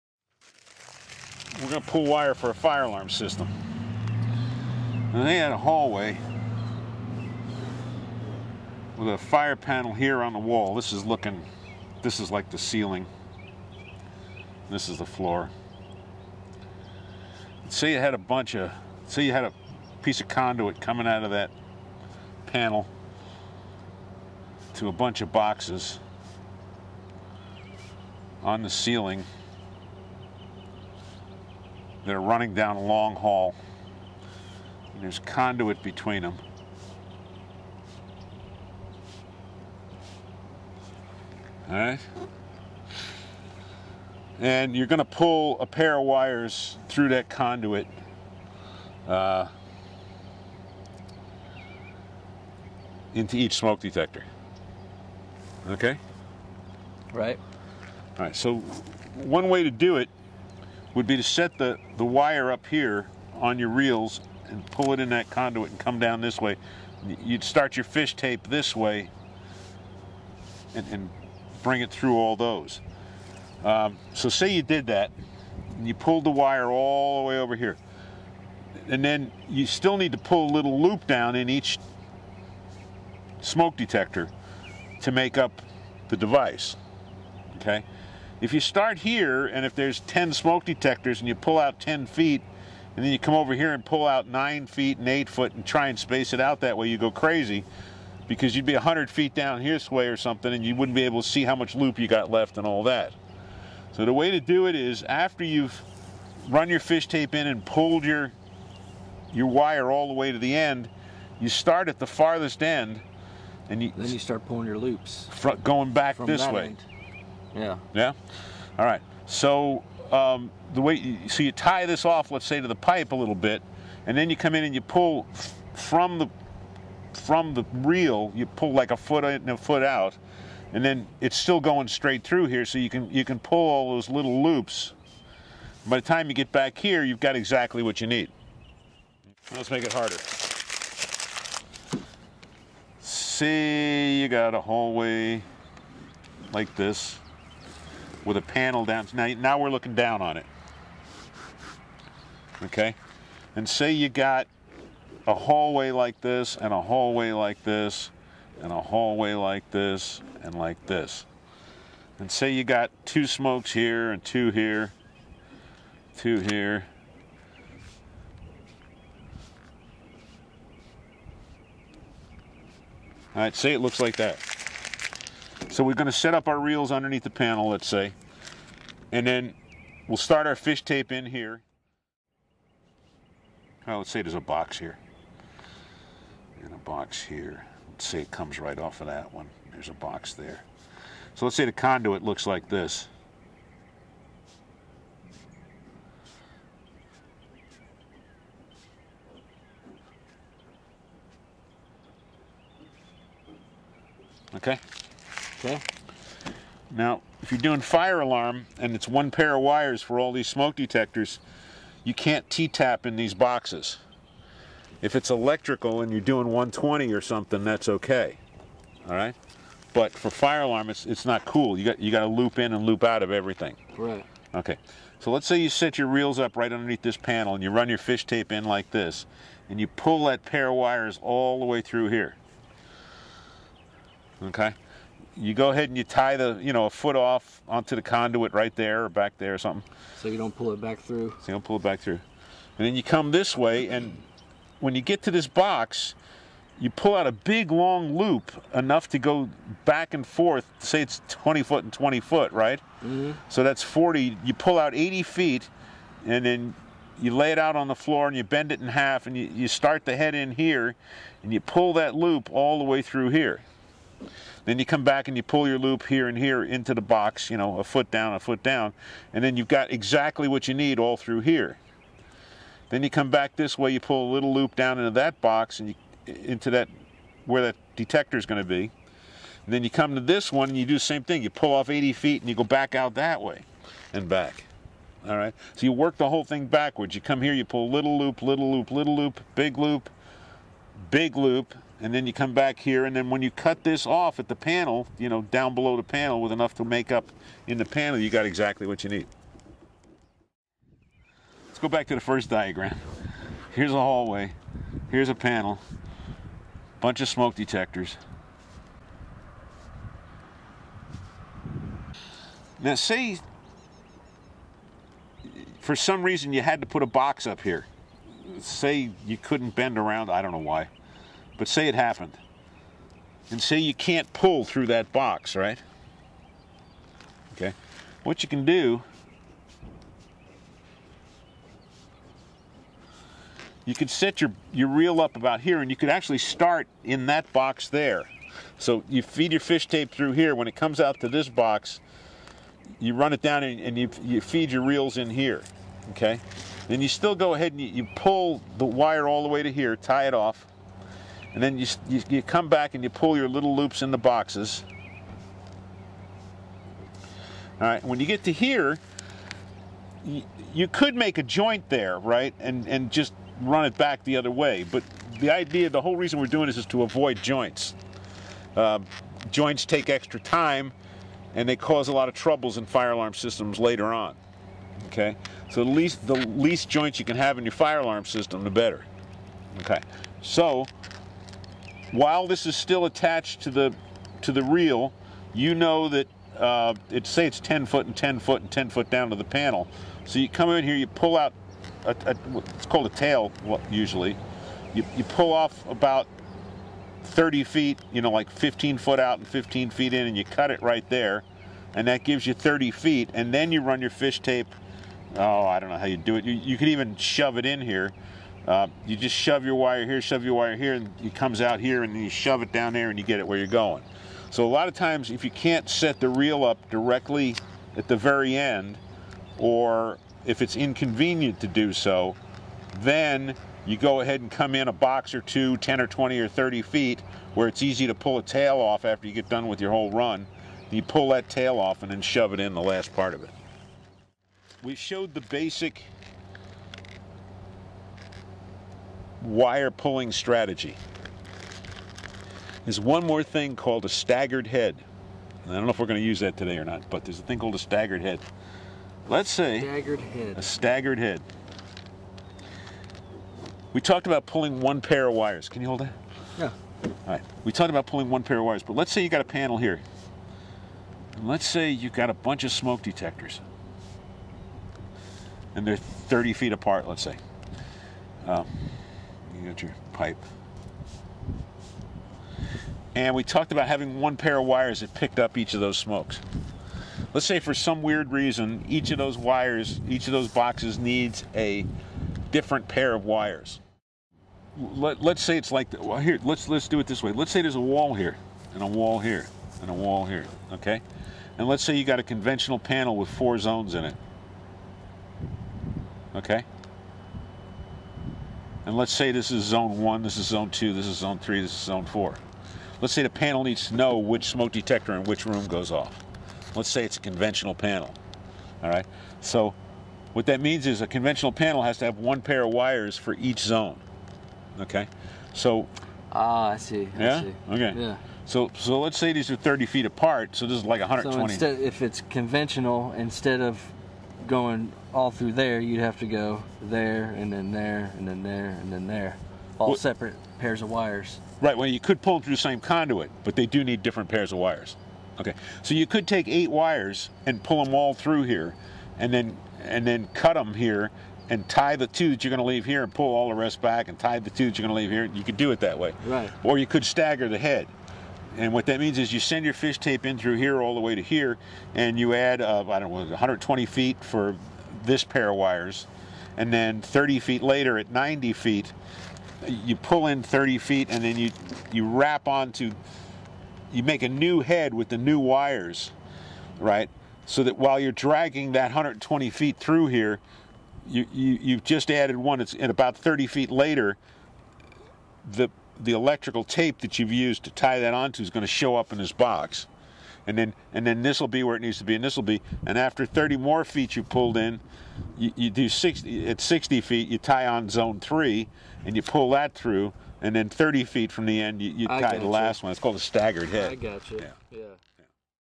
How to pull wire
This video shows the correct way to pull wire in a fire alarm system in conduit. But also applies to free wire systems and 120 V. systems.

Special thanks to Stephen Hamerla of Atlanta, GA.